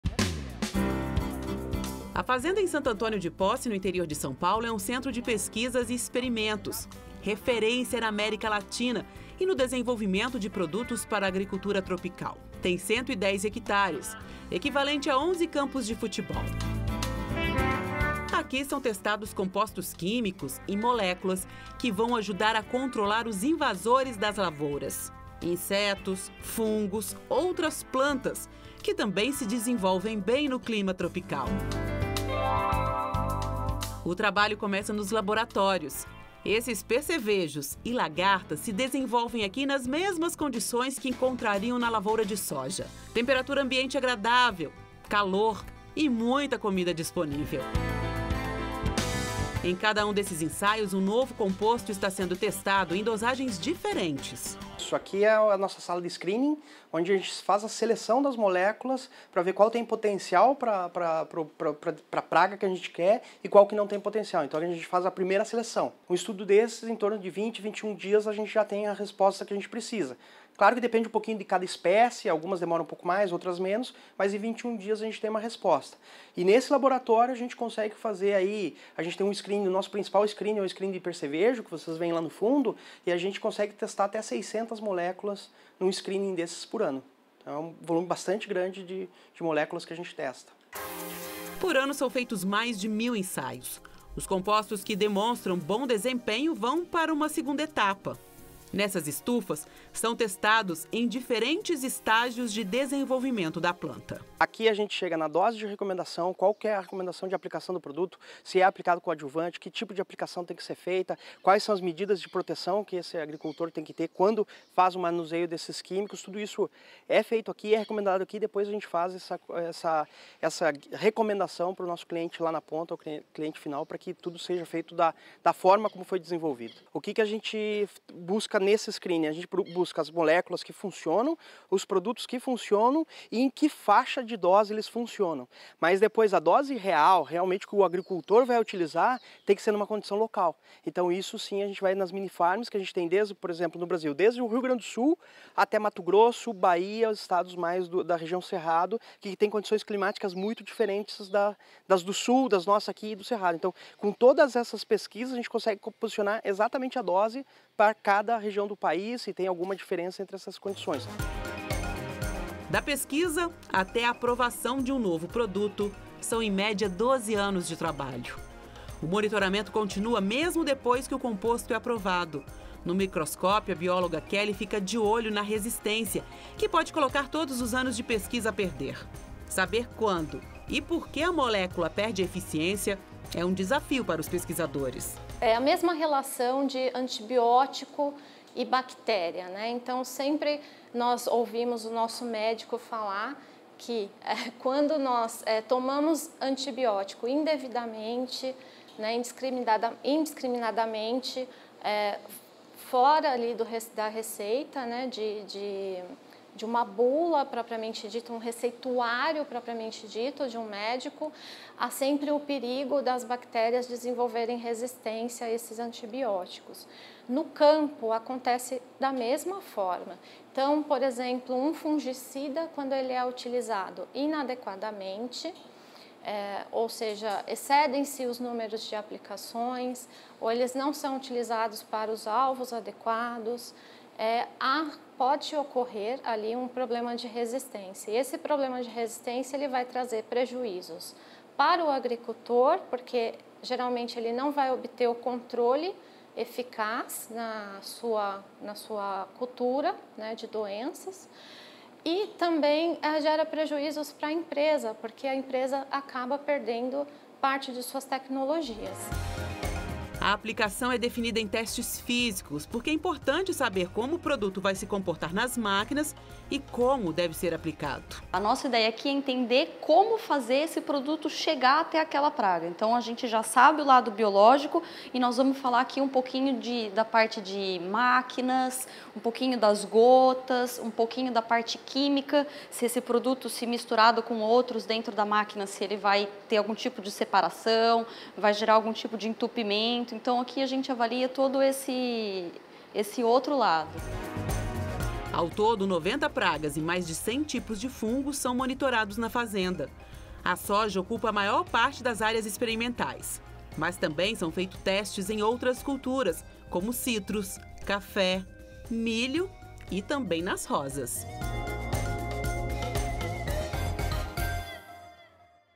Como são desenvolvidos e testados os agrotóxicos no Brasil
Visitamos a fazenda experimental da Basf onde são desenvolvidas e testadas novas moléculas de agroquímicos.

O desafio dos cientistas em conhecer as pragas e as doenças do comuns no clima tropical.